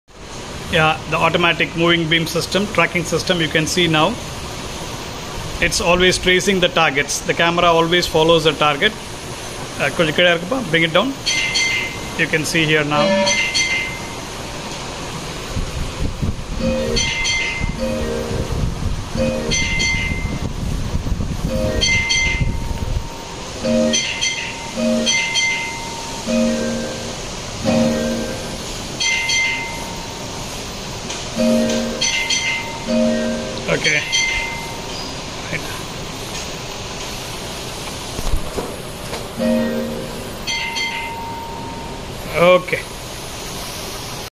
REC - WA400 AT - 3D WHEEL ALIGNER WITH AUTOMATIC TRACKING SYSTEM
REC WA400 - 3D WHEEL ALIGNER with auto tracking system to be used along with lifts fur wheel alignment.